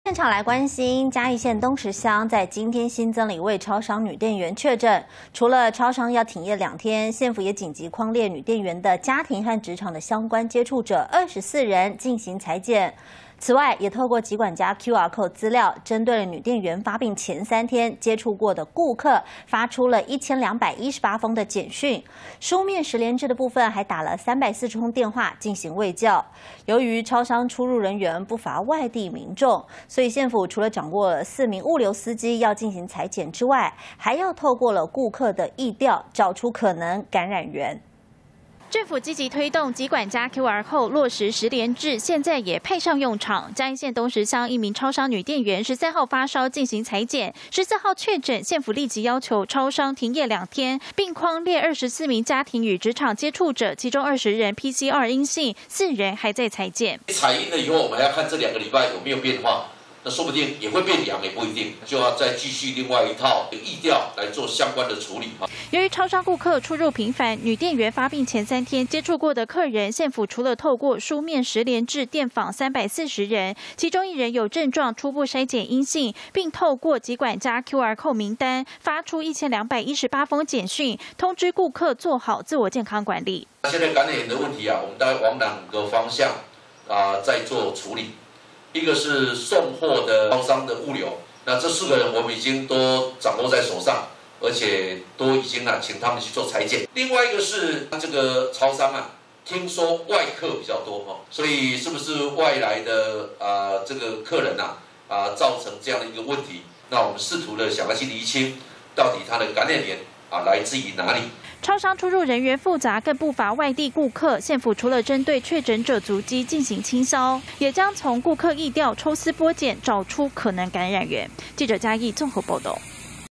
嘉義超商女店員染疫 縣府急發1218封細胞簡訊
嘉義縣東石鄉15日新增一名超商女店員確診，除了超商停業兩天，縣府也緊急匡列女店員的家庭與職場接觸者，一共24人進行採檢。另外也透過疾管家QRCode資料，針對女店員發病前三天，接觸過的顧客，發出1218封簡訊，書面實聯制部分，也打了340通電話進行衛教。由於超商出入人員不乏外地民眾，縣府除了掌握4名物流司機進行採檢，也將透過顧客疫調，找出可能感染源。